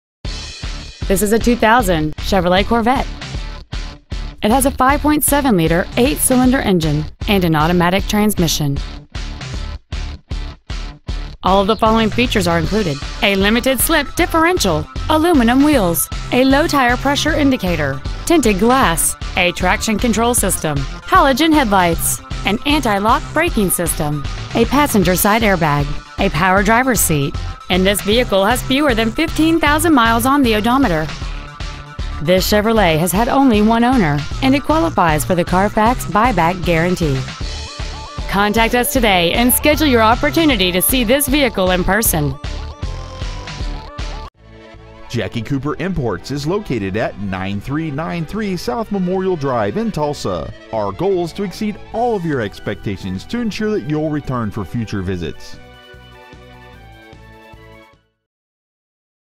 2000 Chevrolet Corvette Tulsa OK 74133
Year : 2000
 Make : Chevrolet
 Model : Corvette
 Engine : 5.7L (346) SFI V8 aluminum engine
 Trans . : Automatic
 Exterior : Green
 Miles : 14,446
 Interior : Other
 Stock : Y5126058